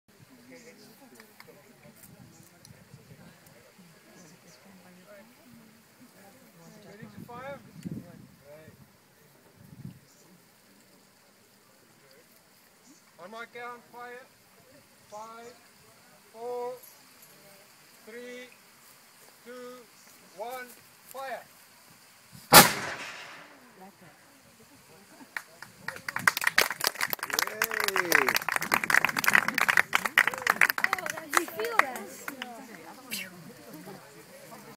Fired a 300 year old canon today...
Canon at Ke-Monate, just outside Durbanville.